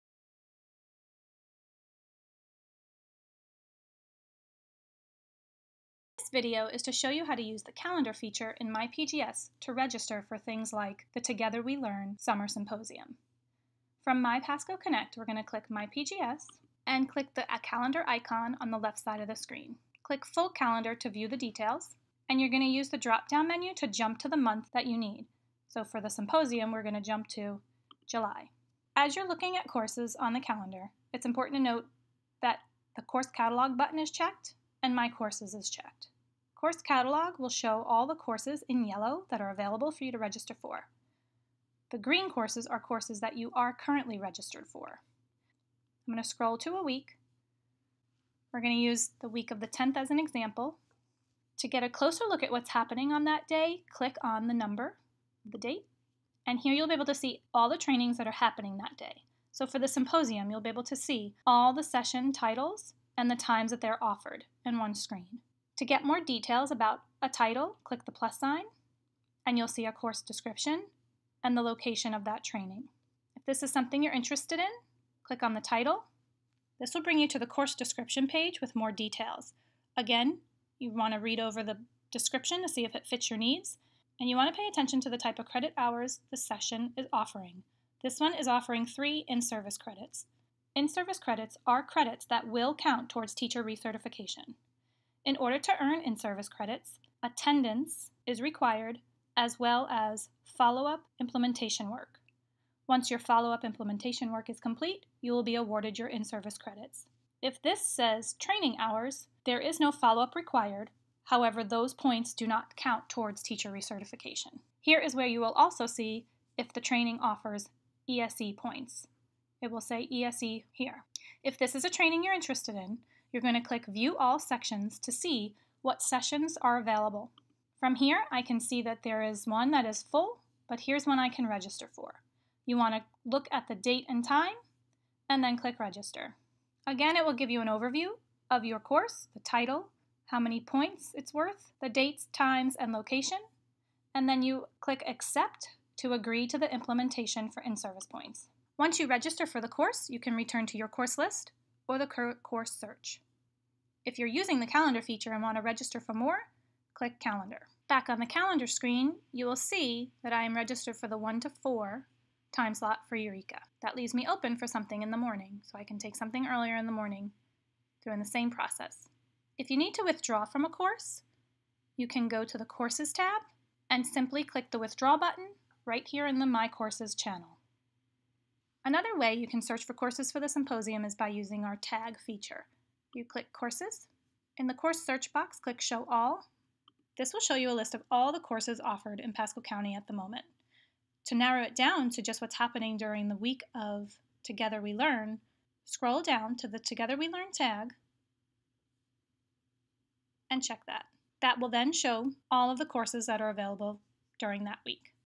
MyPGS: How to use myPGS to register for an event within Pasco County Schools
Tutorial that walks you through registering with the use of a calendar with myPGS.